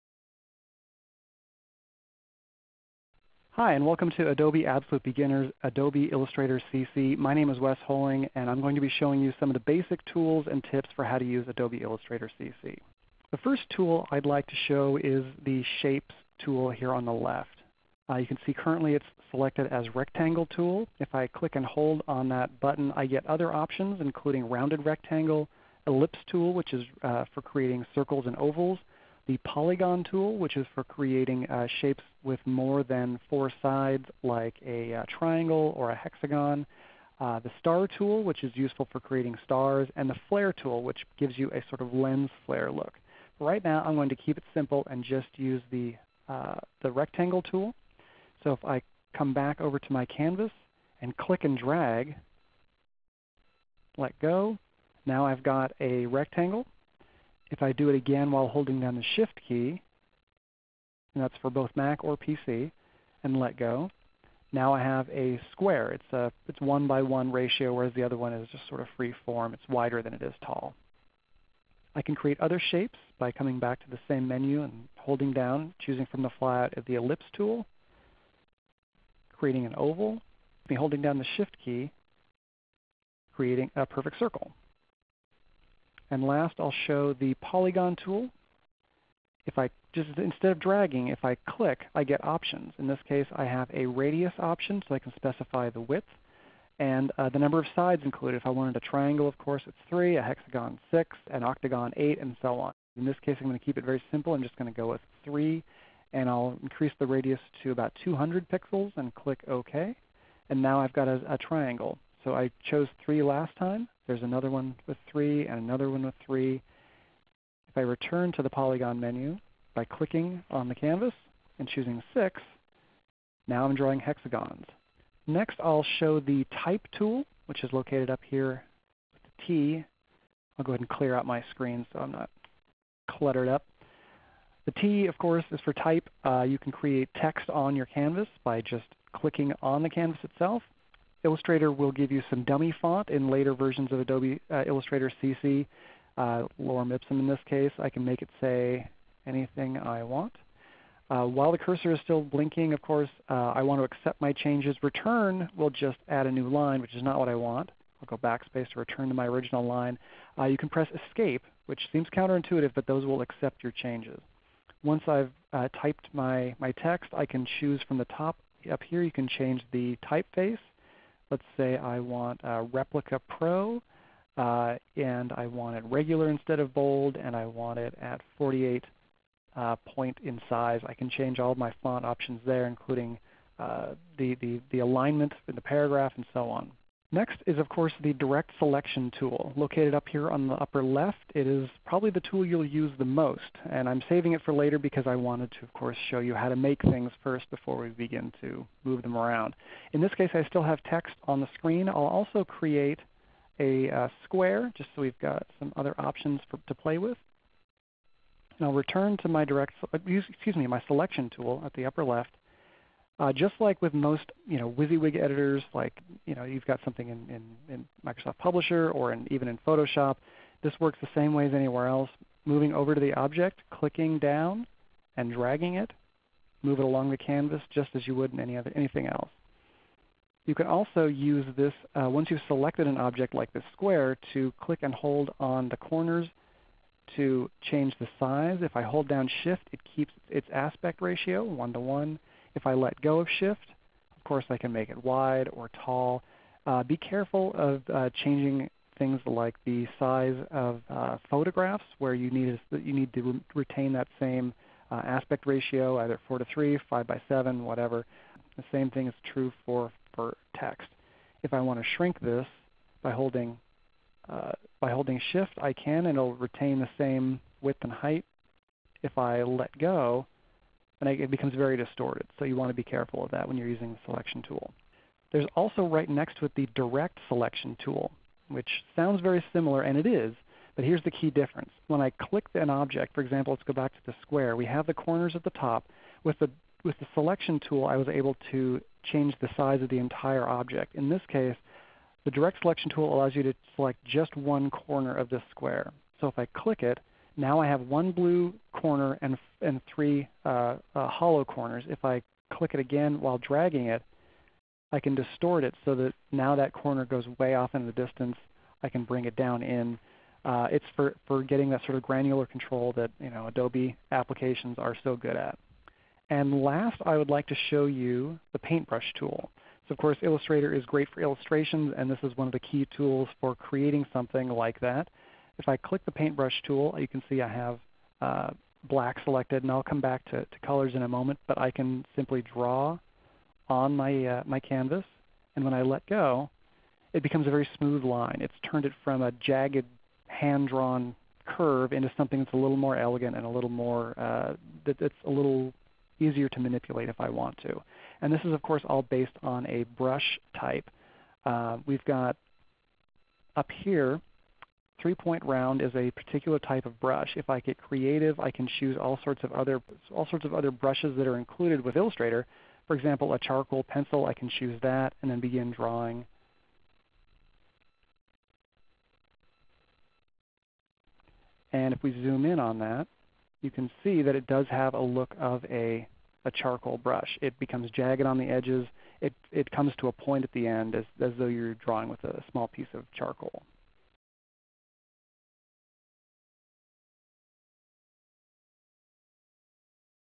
Adobe - Tutoriel "bien débuter avec Illustrator" - les 5 outils indispensables 2017 VOSTFR
[Pensez à activer les sous-titres en bas à droite [CC] de votre fenêtre]
Une courte vidéo de tutoriel en VOSTFR pour découvrir les 5 outils indispendable sous Illustrator